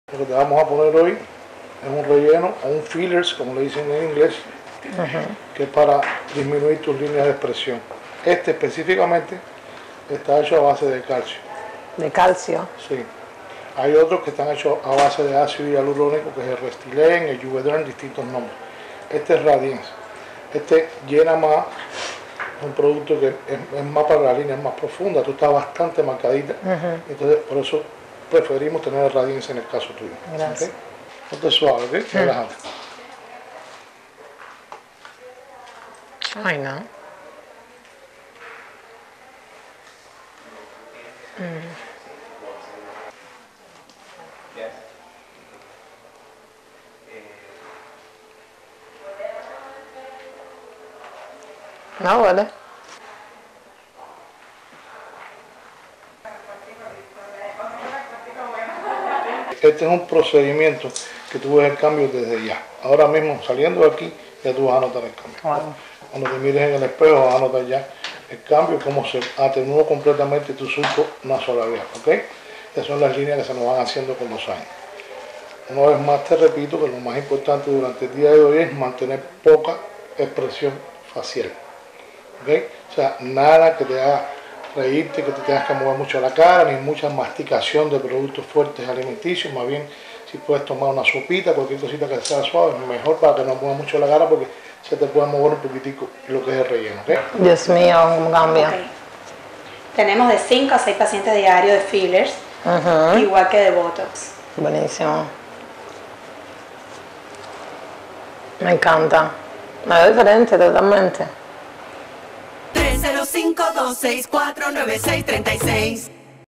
Aplicación de Radiesse en My Cosmetic Surgery Miami
Yuliet llega hasta My Cosmetic Surgery Miami buscando relleno para  las arrugas y líneas de expresión facial. Y después de la consulta con el Doctor Rogelio Iglesias decide aplicarse Radiesse.
Mediante la inyección de Radiesse a nivel de la piel del óvalo facial, para recuperar su definición. De esta forma desapareció la flacidez generada con el paso de los años en la cara, sin perder en ningún momento la naturalidad en la expresión facial.